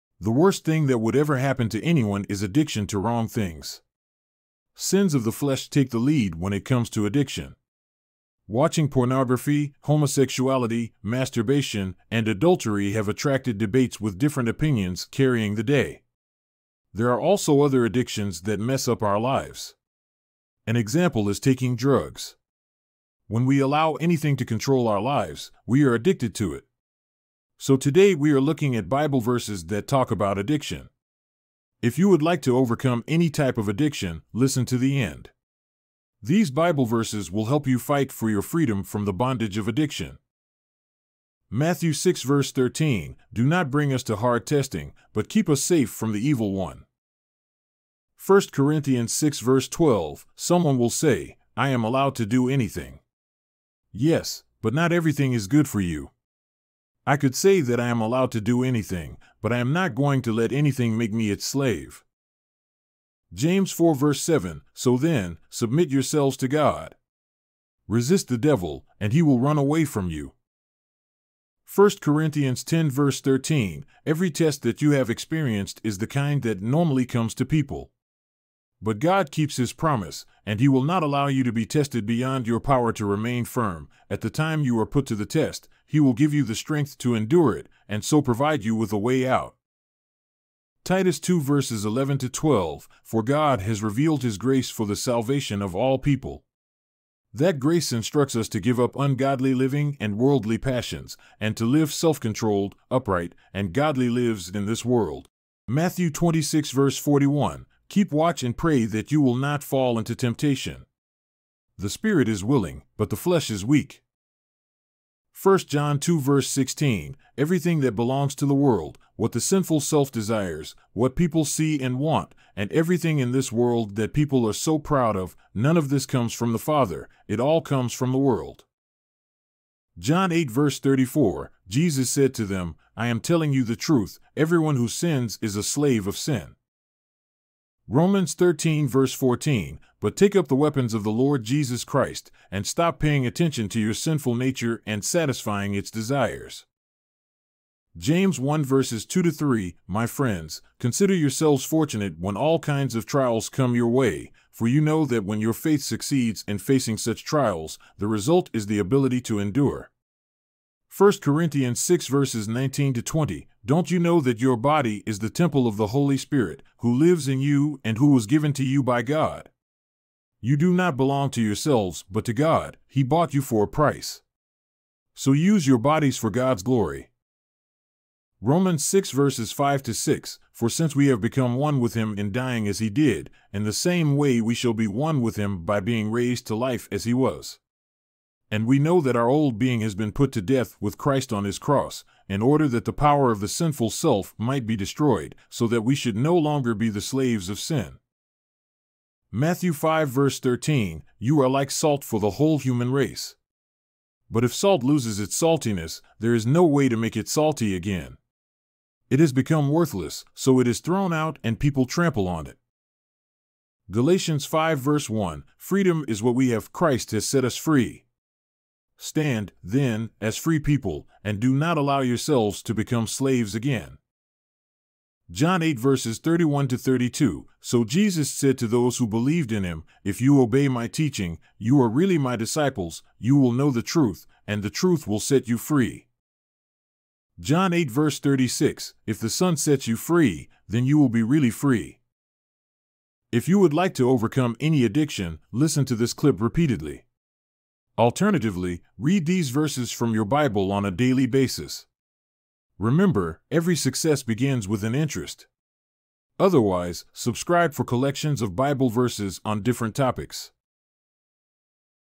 DANGERS OF ADDICTION AND HOW TO OVERCOME IT
Addiction to anything can be disastrous. Addicts may not know how to get out of their addiction without guidance.

For this reason, we came up with this video that has "overcoming addiction Bible verses" to help addicts find their way out of their addiction.

Listening to these Bible verses often will help you understand what God expects of you. Towards the end, you will find Bible verses that reassure you that you are not rejected but rather accepted and loved.